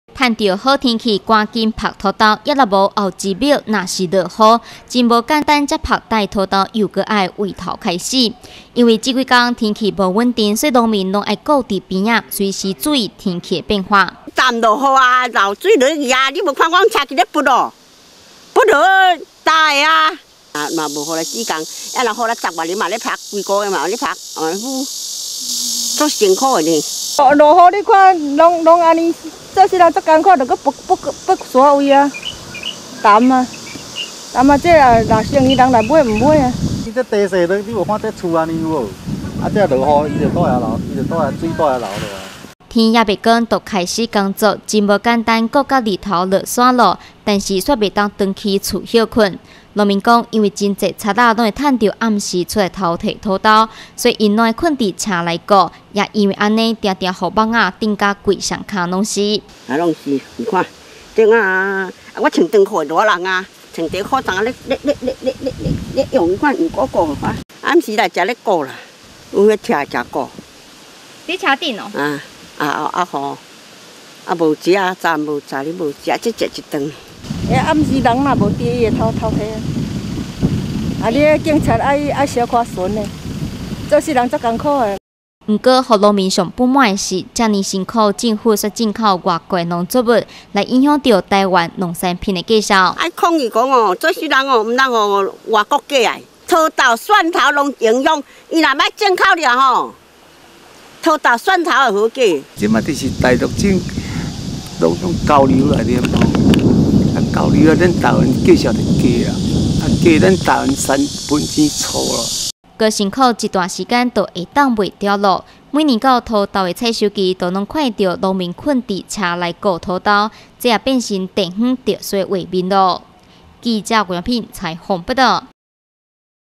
雲林新聞網-台西曬花生
這幾天天氣不穩，有時明明太陽熾熱，不過下一秒卻突然下大雨，這無法預測的天氣讓農民叫苦連天，尤其是非常需要曬太陽的花生更不能淋雨，所以農民是整天顧在一旁隨時看老天爺的臉色，雖然現在價格不是很高，不過還是有小偷覬覦，所以就算回家睡也不能安心，農民只好以車為家。